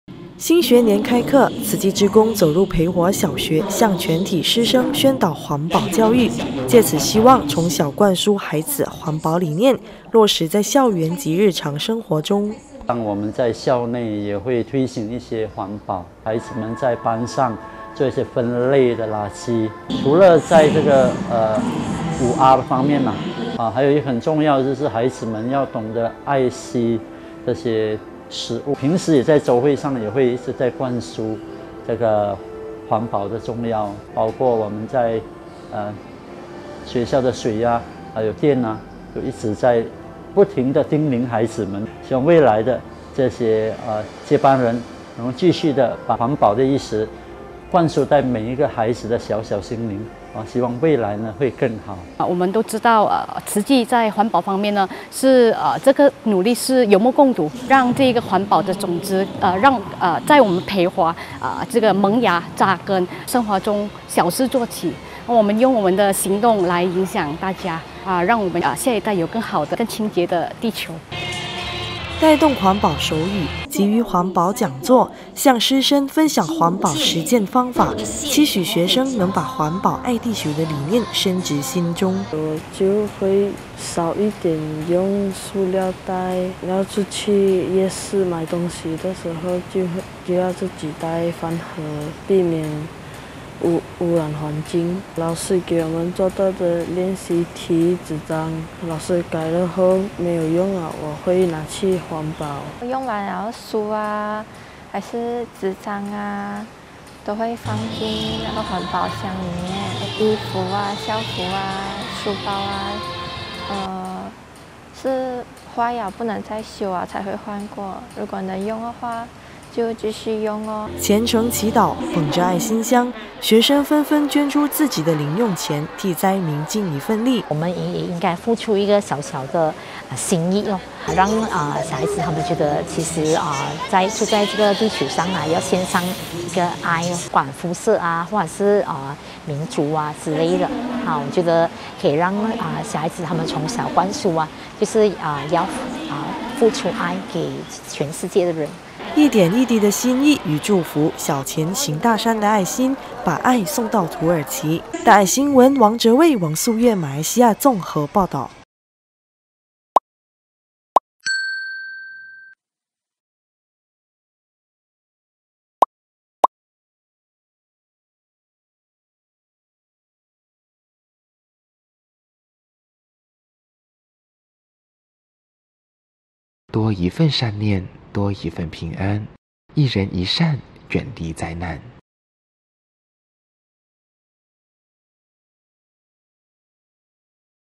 【新聞】 校園環保講座 募善念虔祈福
慈濟吉打分會志工前往培華小學舉辦「環保入校園」活動，向全體師生宣導環保理念，分享環保生活化。同時，志工帶領全體師生虔誠祈禱為土耳其災民，募愛心，啟發善念。

馬來西亞
吉打

校園環保 布善種子
新學年開課，慈濟志工走入培華小學向全體師生宣導環保教育，藉此希望從小灌輸孩子環保理念，落實在校園及日常生活中。

培華小學校長 林亞冬
我們在校內也會推行一些環保
孩子們在班上做一些分類的垃圾
除了在這個 5 R 方面  還有很重要的孩子懂得愛惜這些食物
平時也會在週會上 也一直灌輸環保的重要
包括我們在學校的水 還有電  一直在不停的叮嚀孩子們
希望未來的這些接班人 繼續的把環保的意識
灌輸在每一個孩子的小小心靈  希望未來會更好

老師 曾麗凌
我們都知道慈濟在環保方面 這個努力是有目共睹
讓這個環保的種子在我們培華萌芽扎根
從生活中小事做起  我們用我們的行動影響大家
讓我們下一代有更好的  更清潔的地球

宣導愛護地球 環保觀念植心田
帶動環保手語、給予環保講座、向師生分享環保實踐方法，期許學生能把環保愛地球的理念深植心中。

學生  柯翰宏
我就會少一點用塑料袋  要出去夜市買東西的時候
就要自己帶飯盒避免 避免污染環境
老師給我們做的練習題紙張 老師改了後  沒有用了 我會拿去環保

學生  葉宜萱
用完的書還是紙張都會放進環保箱
衣服 校服 書包 是壞了不能再修了 才會換過 如果能用 就繼續用

募集善念 為土耳其祈福
虔誠祈禱、捧著愛心箱，學生紛紛捐出自己的零用錢，替災民盡一分力。

老師 傅峰銀
我們也應該付出一個小小的心意
讓小孩子覺得其實住在這地球上要獻上一個愛
不管膚色或者是民族之類  可以讓小孩子從小灌輸  就是付出愛給全世界的人

點滴愛心 化為祝福
一點一滴的心意與祝福，小錢行大善的愛心，把愛送到土耳其。

馬來西亞綜合報導
王哲蔚 王素月